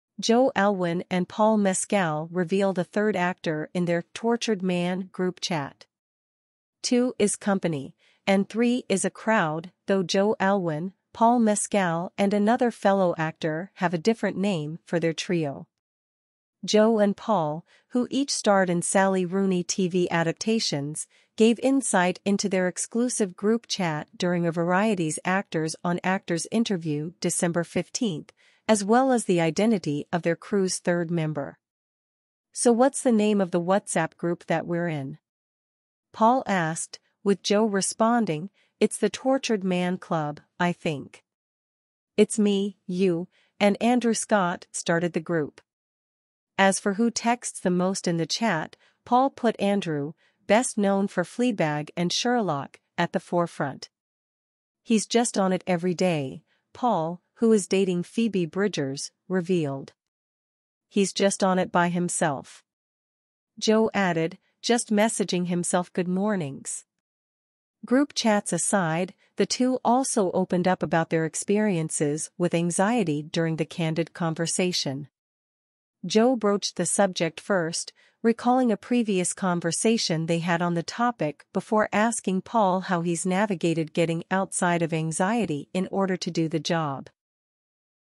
Joe Alwyn and Paul Mescal Reveal the 3rd Actor in Their “Tortured Man” Group Chat | EL!News
Joe Alwyn and Paul Mescal Reveal the 3rd Actor in Their “Tortured Man” Group Chat
Joe Alwyn and Paul Mescal gave insight into their WhatsApp group chat, which was originally created by a certain Fleabag actor.

©NOTE:_Some Images, Music, Videos, and Graphics, are shown in this video May be Copyrighted to respected owners, not mine